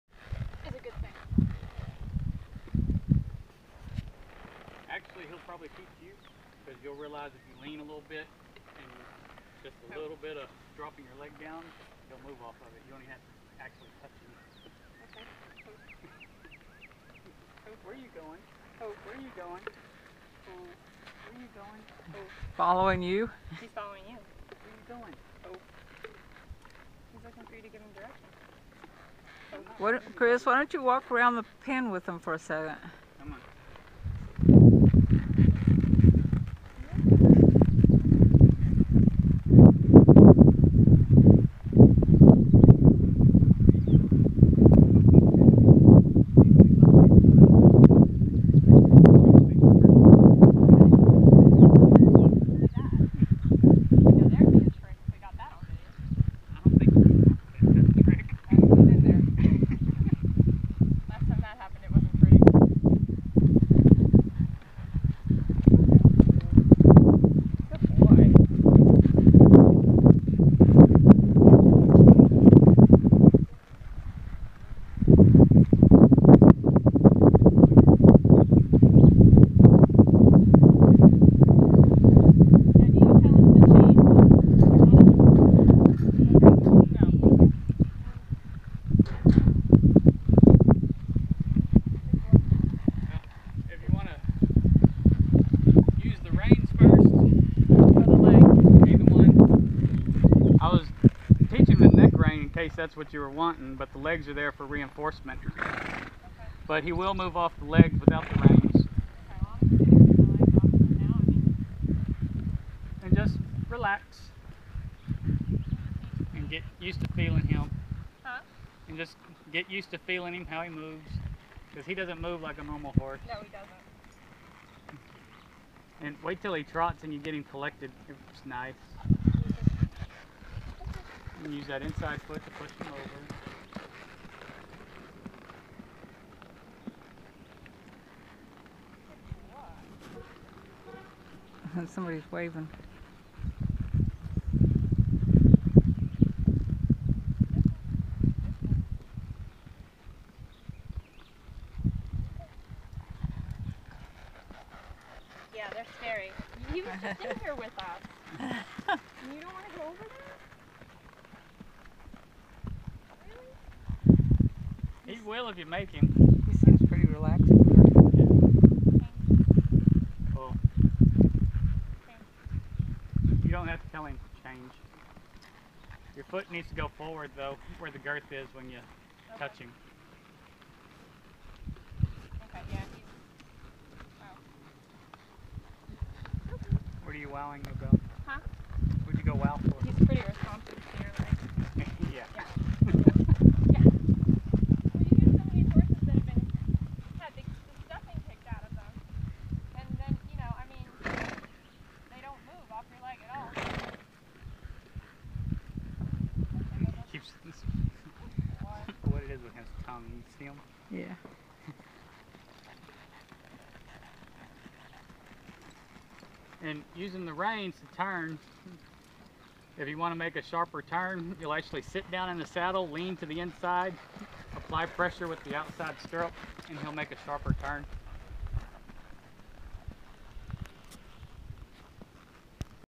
Kristen riding Toby for the first time
This is the first time Kristen has gotten to ride Toby